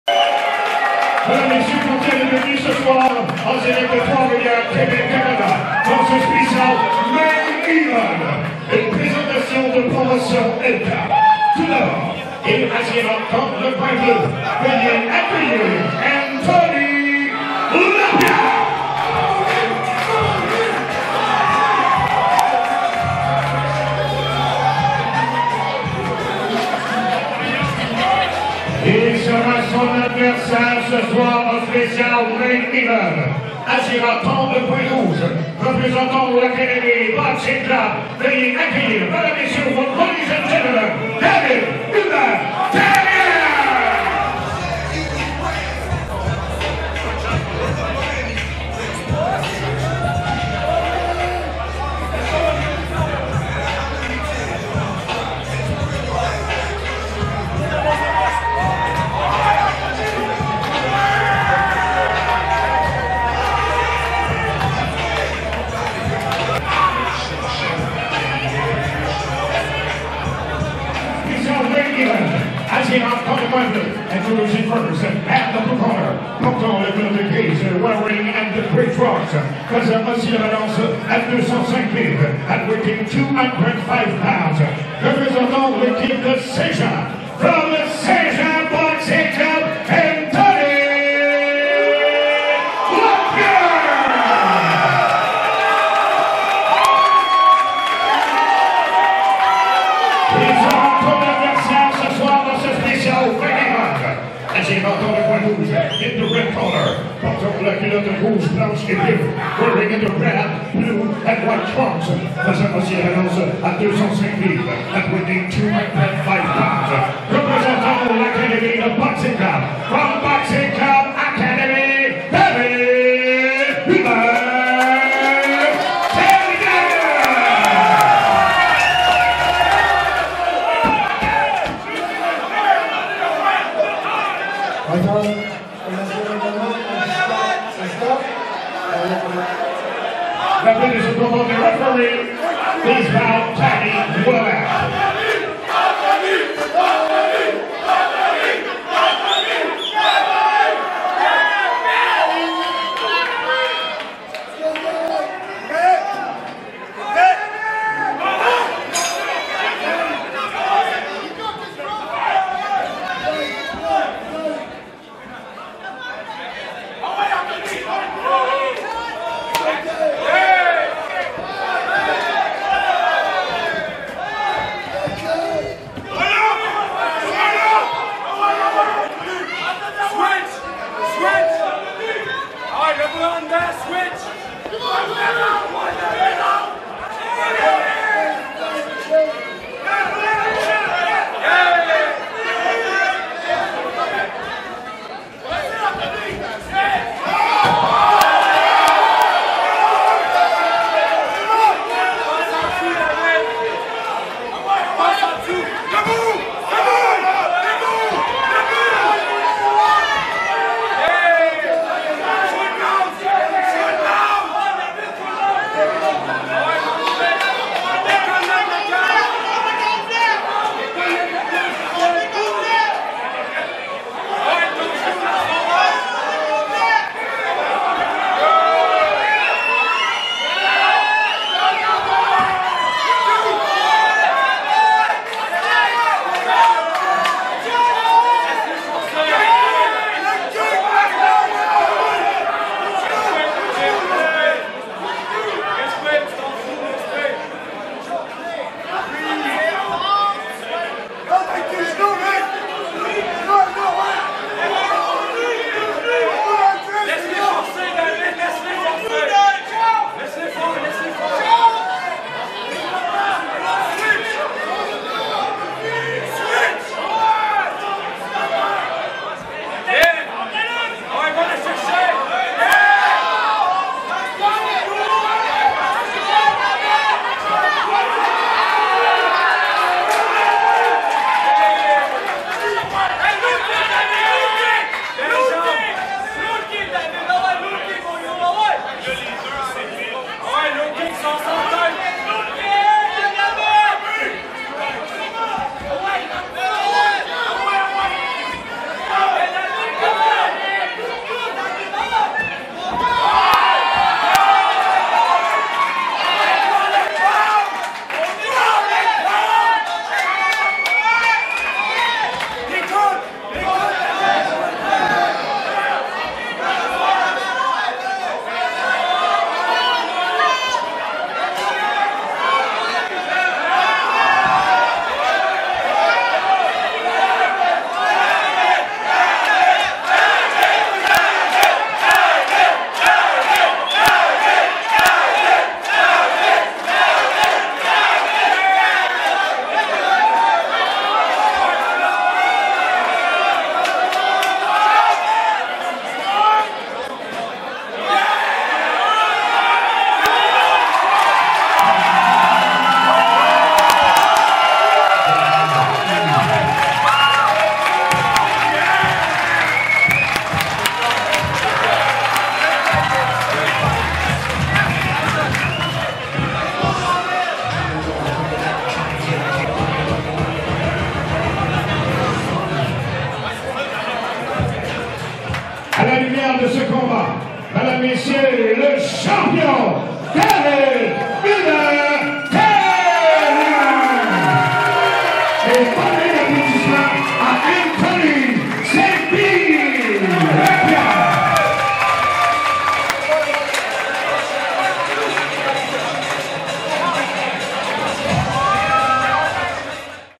ANTHONY LAPIERRE vs DAVID HUBERT THERRIEN
Anthony Lapierre affronte David Hubert Therrien en combat mma, lors de la soirée DFC 5: THE NIGHT OF CHAMPION, soirée de combats qui s'est tenu à l'hippodrome de Trois-Rivières, Québec, Canada, le 31 mars 2018.